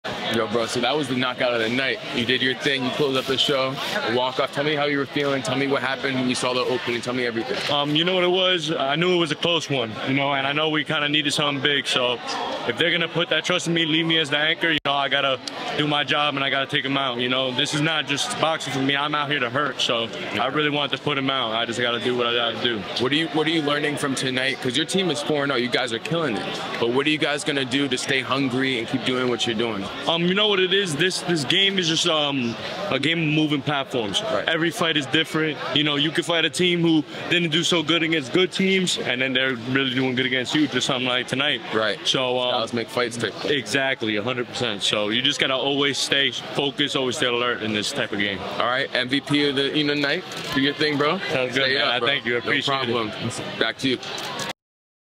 Ralph Clemente KO Artist
Ralph Clemente of team New York Attitude sealed the deal to help keep their team undefeated for the season by knocking down Kennth Peoples twice in the 18th and final round.